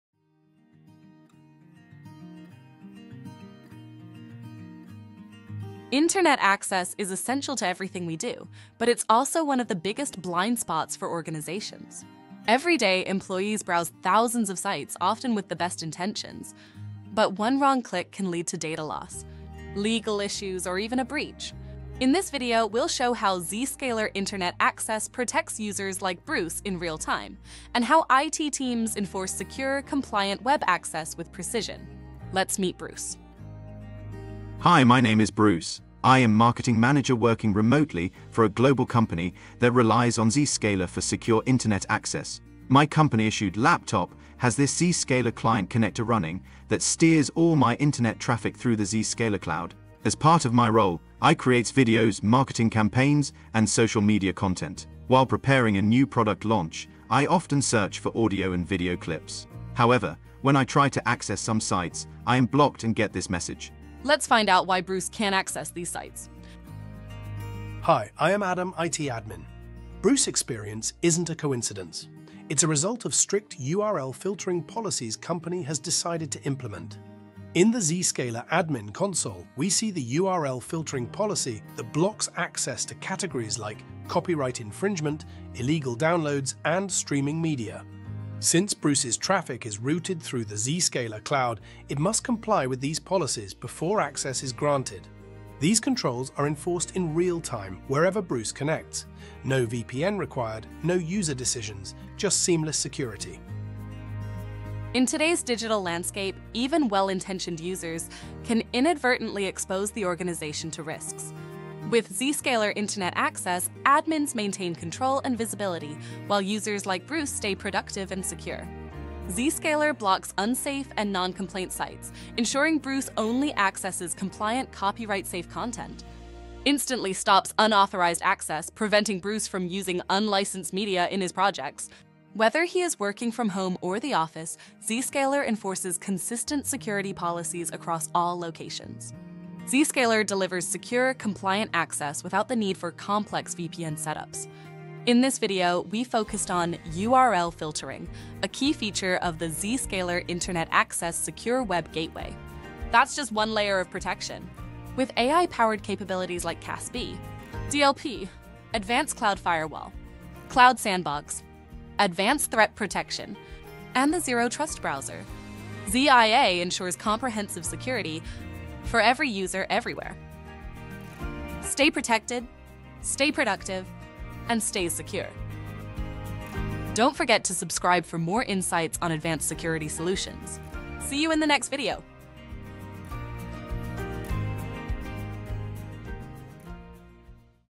ZIA URL Filtering
Zscaler URL Filtering delivers seamless internet security by blocking malicious, unsafe, and non-compliant websites — keeping users protected without the hassle of traditional VPNs. In this video, we explore how Zscaler Internet Access enforces consistent security policies across all locations, ensuring safe browsing for every user.